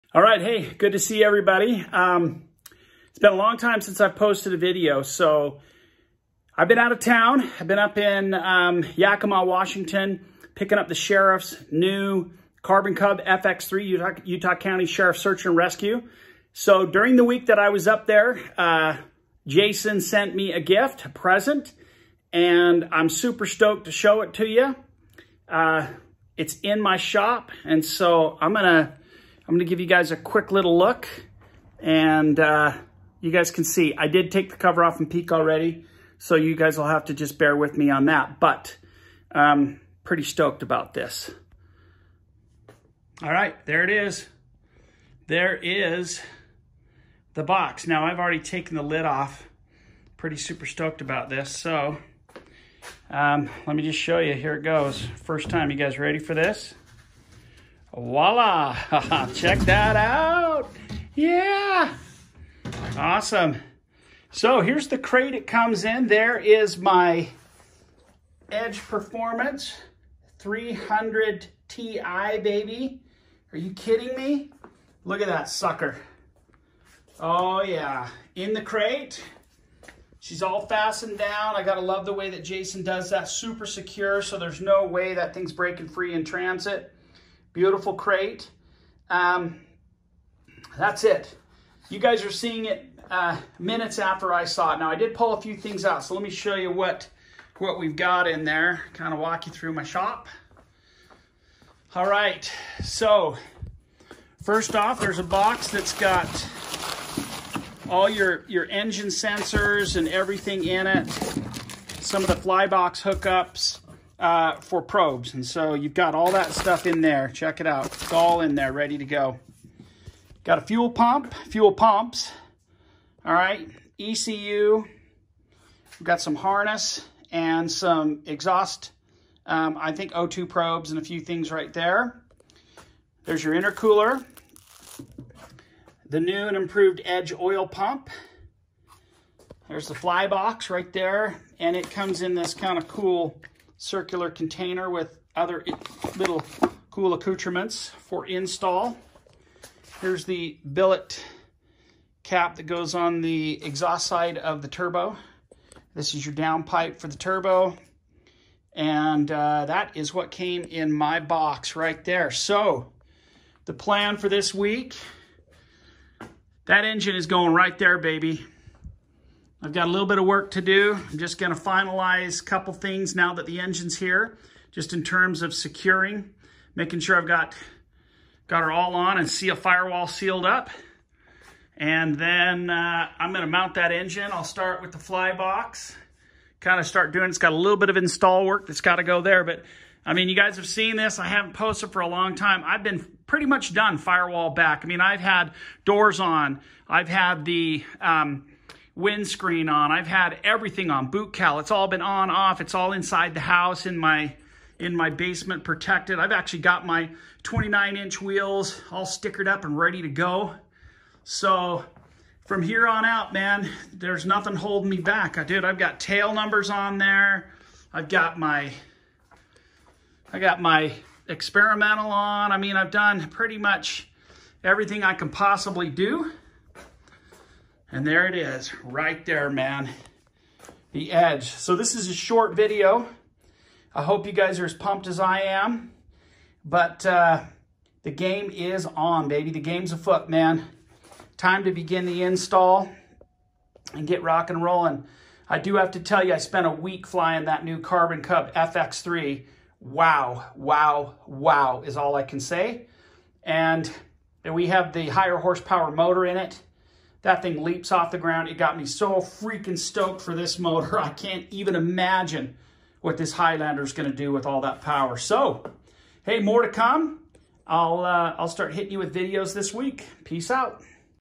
My Edge Apex 300TI Has Arrived!!
The day has finally come!  My Edge Performance Apex has arrived!  Here's a quick look and a sneak peak at whats about to happen.